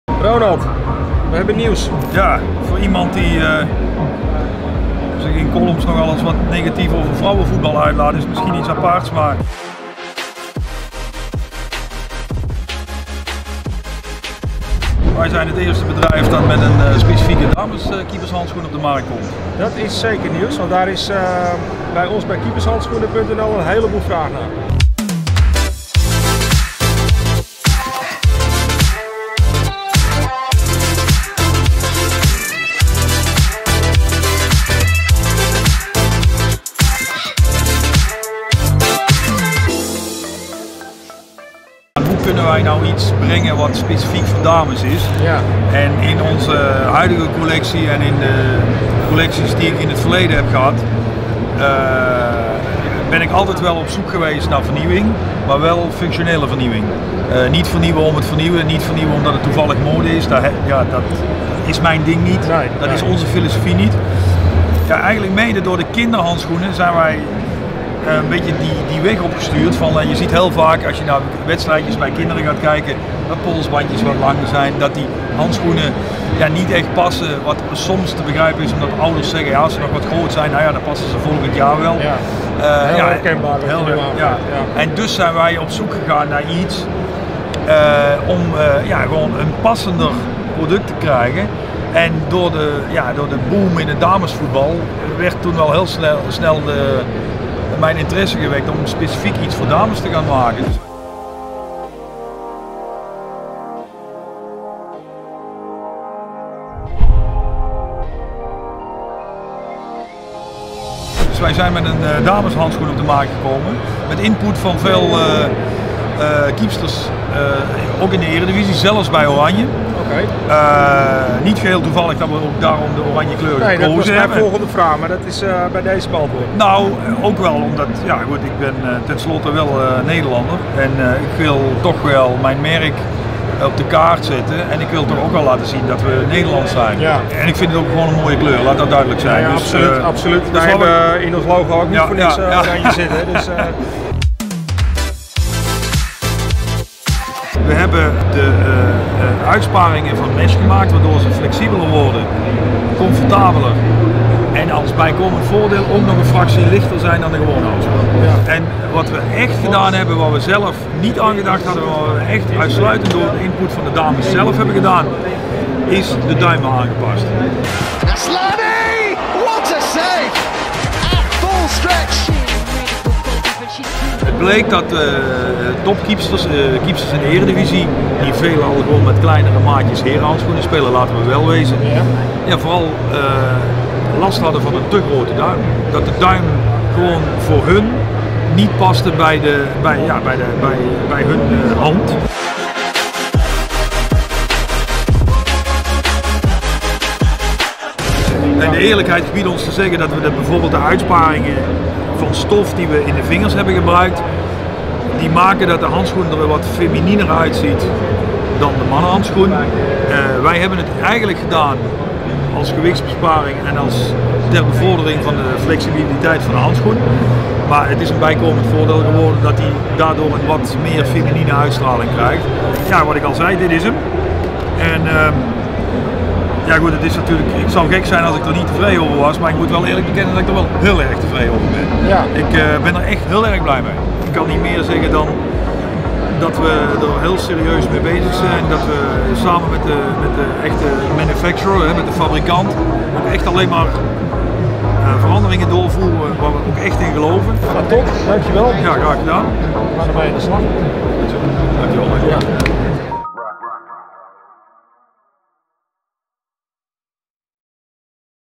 RONALD WATERREUS ONTWERPT SPECIFIEKE RWLK DAMES KEEPERSHANDSCHOENEN
De RWLK Ladies First is de eerste Keepershandschoenen die specifiek voor dames keepers ontworpen zijn. Deze handschoenen zijn ontworpen door oud-keeper Ronald Waterreus. Dit zijn tevens de handschoen waar Jeroen Zoet van PSV mee speelt. Het absolute top materiaal wat er te verkrijgen is. Met deze handschoen heb je een uitstekende grip en gevoel over de bal. Handschoen is gemaakt van neopreen materiaal en heeft een witte binnenhand van 3,5 mm en 3 mm padding. De verstevigde polssluiting is gemaakt van latex en 7,5 cm neopreen. Met de elastiekband is de sluiting naar wens te verstellen. Eigenschappen: Slim fit, Ultra soft foam, negatieve cut met oranje mesh voor extreem confort.

Extra informatie

Model Ladies First
Maat 6, 7, 8, 9
Doelgroep Junior, Senior
Merk RWLK
Techniek (palm) Negatief Naad
Productlijn Nee
Ondergrond Gras

Videomontage:
11 Shots Media